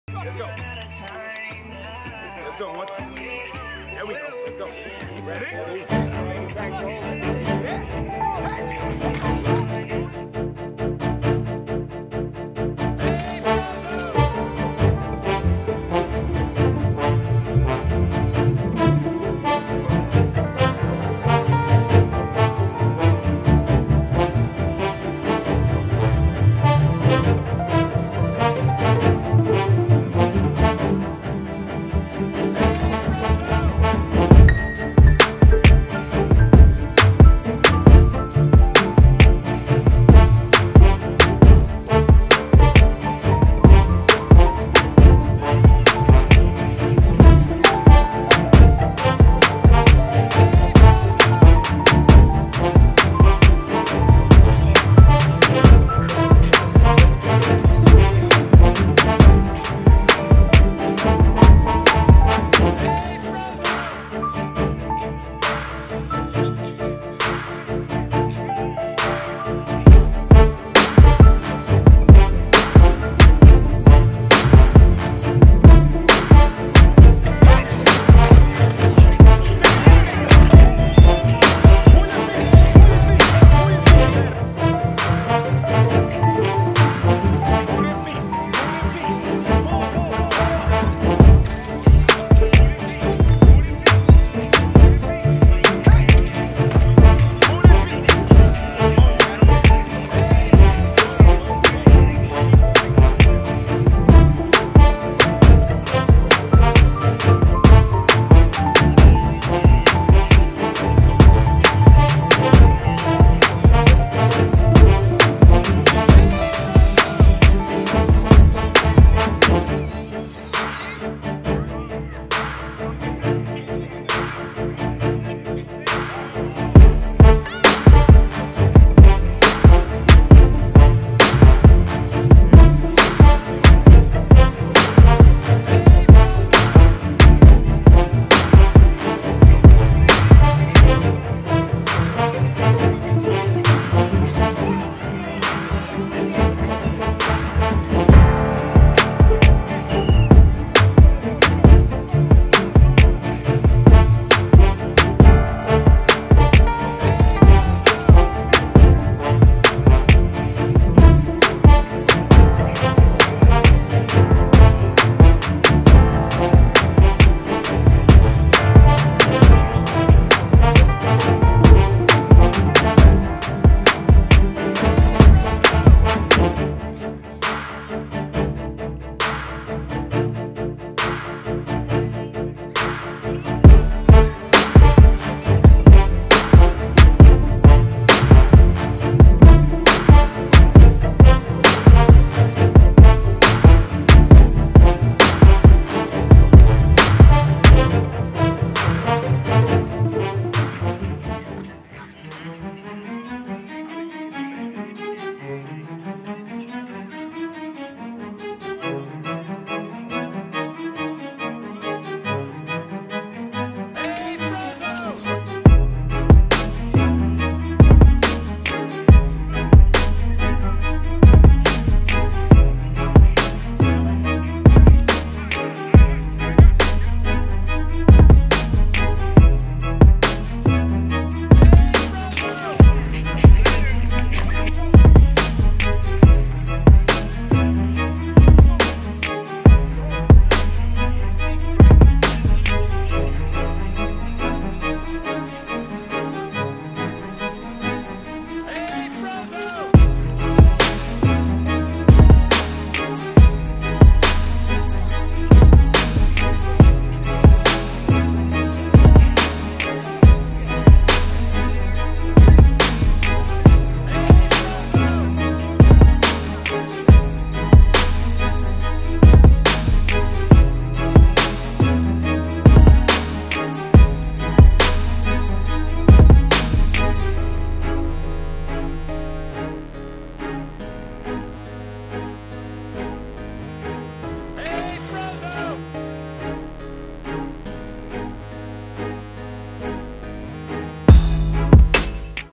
🔥🔥 The Laced Facts Experience | Day 2 | Under The Radar
🔥🔥 The Laced Facts Experience | Day 2 | Under The Radar  | 2017

A showcase featuring some of the nation's best youth football players. The Laced Facts Experience took place at  El Camino Real High in Southern California. Mike Evans, former D1 baller brought together many elite young ballers to compete with the best of the best and showcase their skills.